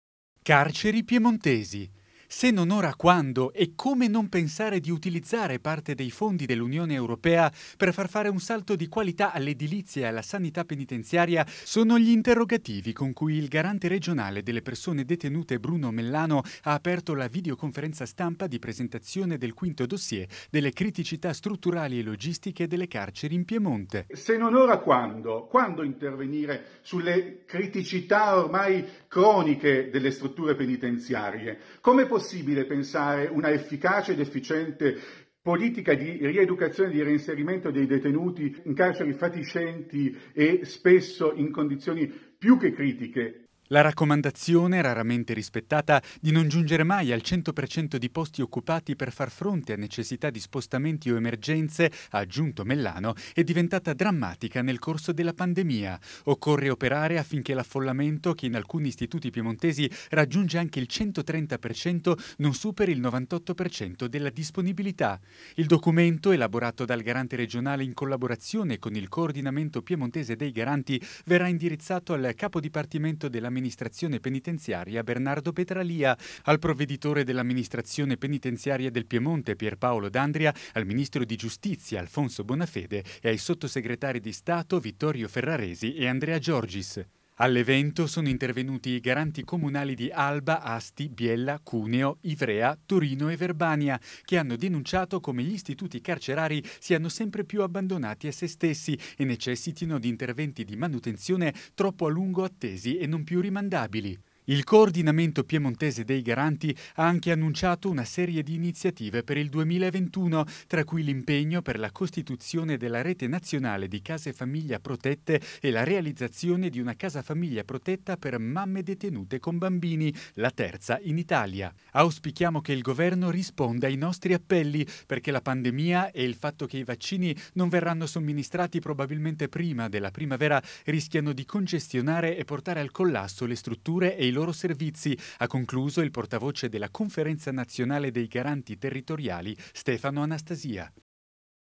Dossier carceri piemontesi
"Se non ora quando? Come non pensare di utilizzare parte dei fondi Ue destinati all’Italia per far compiere un salto di qualità alla sanità e all’edilizia penitenziaria?”. Questo il doppio interrogativo esposto dal garante regionale delle persone detenute Bruno Mellano, durante la videoconferenza stampa di presentazione del Quinto dossier delle criticità strutturali e logistiche delle carceri piemontesi.
Il documento, elaborato dal garante regionale in collaborazione con il Coordinamento piemontese dei garanti, verrà indirizzato al capodipartimento dell’Amministrazione penitenziaria Bernardo Petralia, al provveditore dell’Amministrazione penitenziaria del Piemonte Pierpaolo D’Andria, al ministro di Giustizia Alfonso Bonafede e ai sottosegretari di Stato Vittorio Ferraresi e Andrea Giorgis.